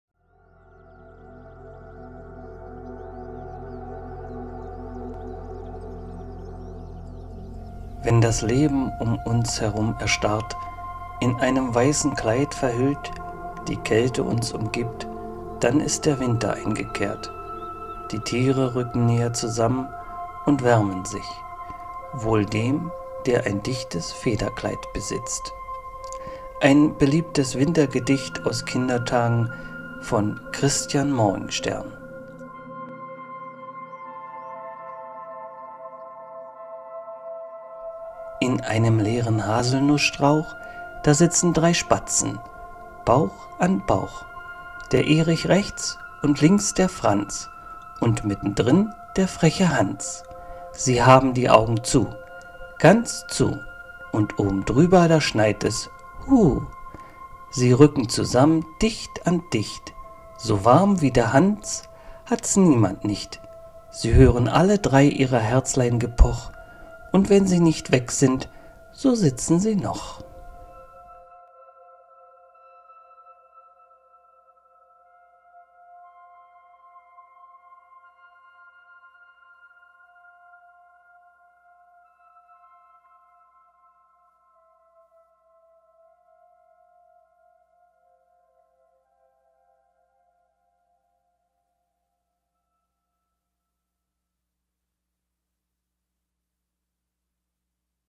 Die drei Spatzen - ein beliebtes Wintergedicht von Christian Morgenstern
Wenn das Leben um uns herum erstarrt, in einem weißen Kleid verhüllt, die Kälte uns umgibt, dann ist der Winter eingekehrt. Die Tiere rücken näher zusammen und wärmen sich. Wohl dem, der ein dichtes Federkleid besitzt.

Ein beliebtes Wintergedicht aus Kindertagen von Christian Morgenstern.

Die drei Spatzen

In einem leeren Haselstrauch
Da sitzen drei Spatzen, Bauch an Bauch.

Der Erich rechts und links der Franz
Und mitten drin der freche Hans.

Sie haben die Augen zu, ganz zu,
Und obendrüber da schneit es, hu!

Sie rücken zusammen dicht an dicht.
So warm wie der Hans hats niemand nicht.

Sie hören alle drei ihrer Herzlein Gepoch.
Und wenn sie nicht weg sind, so sitzen sie noch.